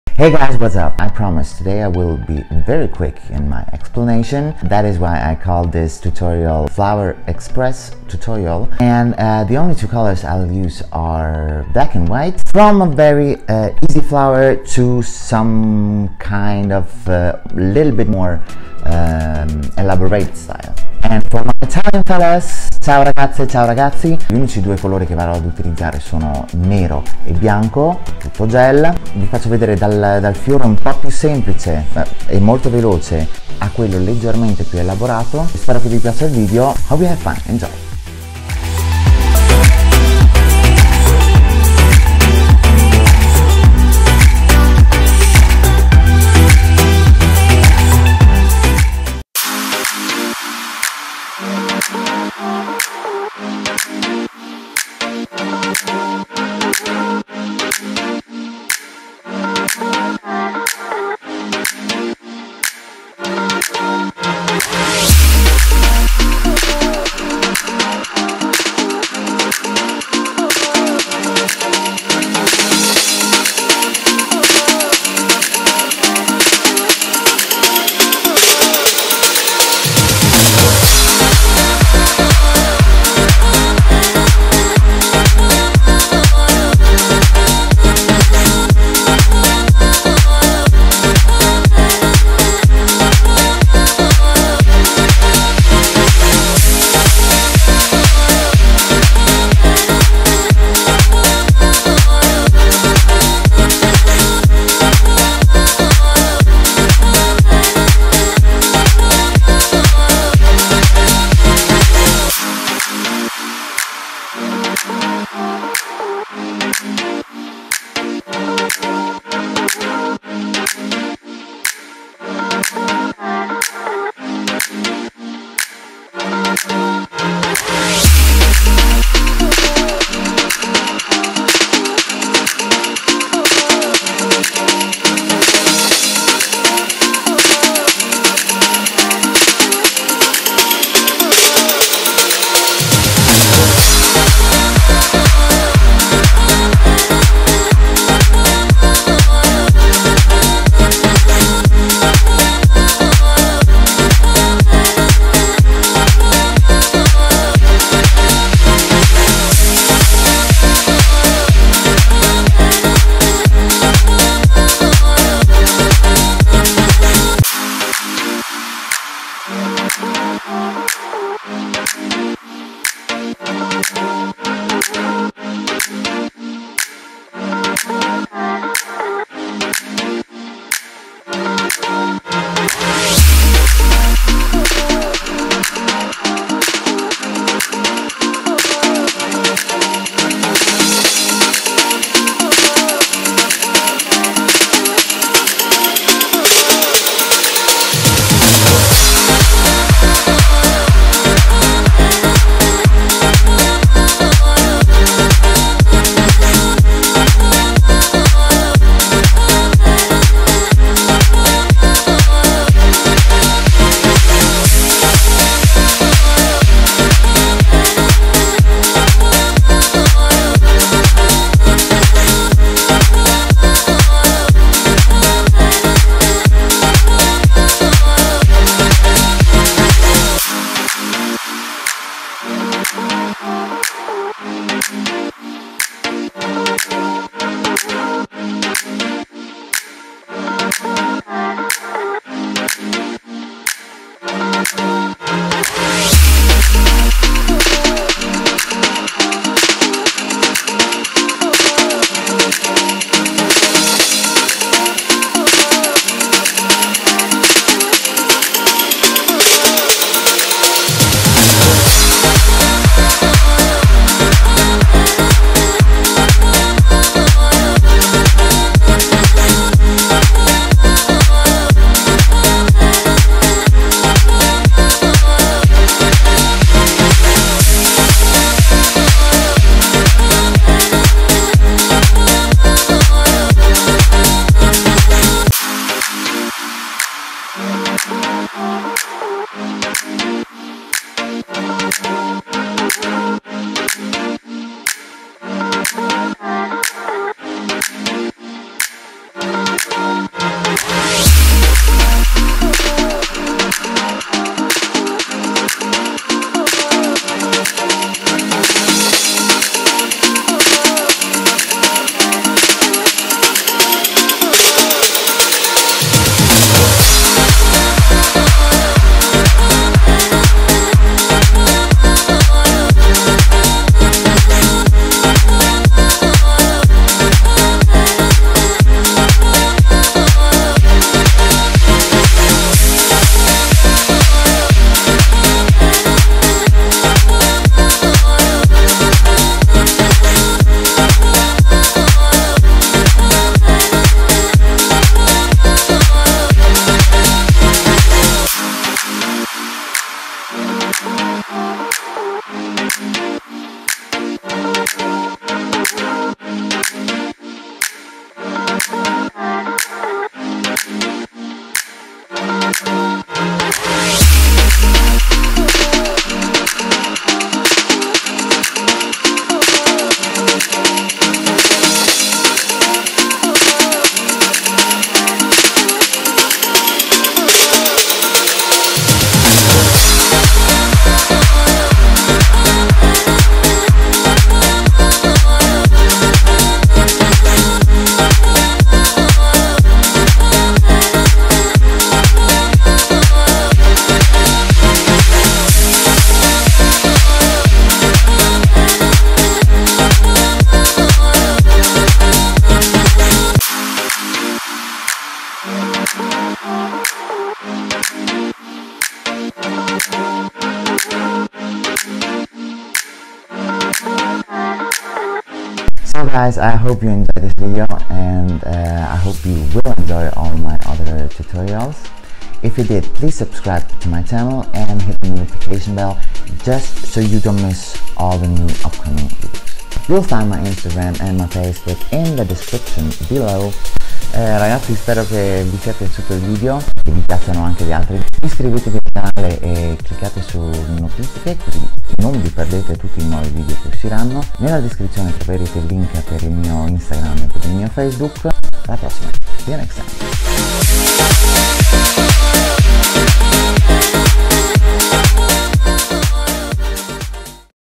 Flower Nail Art Express Tutorial - 4 Designs!
In this tutorial I show you how to make a quick Flower Nail Art! I used different gels and gellacks in white and black. Enjoy and have fun!

In questo tutorial vi faccio vedere come realizzare velocemente una Nail Art floreale. Ho usato gel e semipermanente bianchi e neri. Il marchio che utilizzo io è la KB School. Spero vi piaccia!